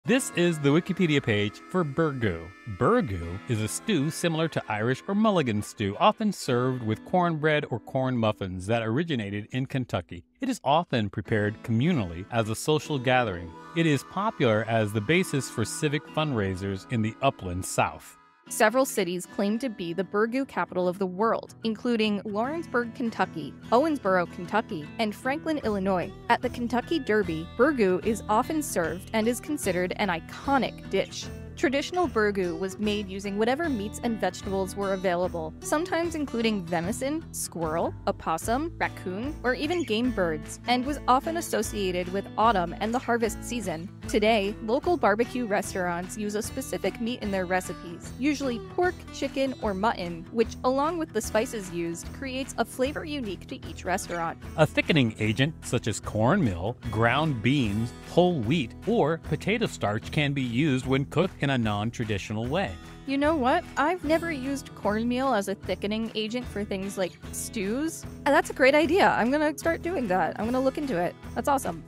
WikiListen: Burgoo
🎧 Get ready for a mouth-watering history lesson! 🍲 Dive into the savory world of Burgoo, the hearty stew with a rich past. Perfect for foodies and history buffs alike! Watch now! 👀